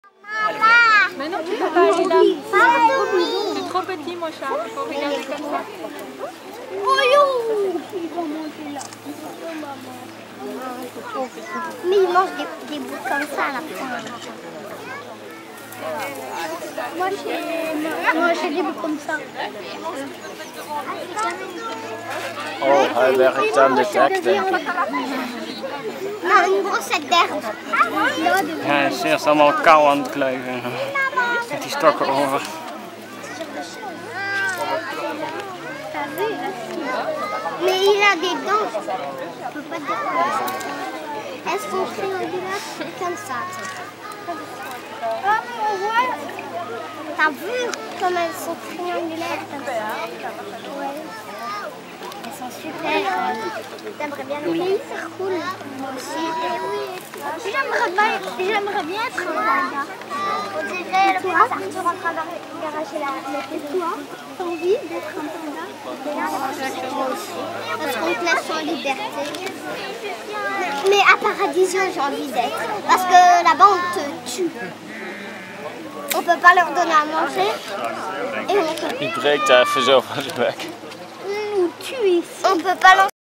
Etende Reuzenpanda 'Hao Hao' in Pari Daiza
Een van de twee Reuzenpanda's in het Belgische dierenpark 'Pairi Daiza', 'Hao Hao' tijdens het eten van wat bamboe.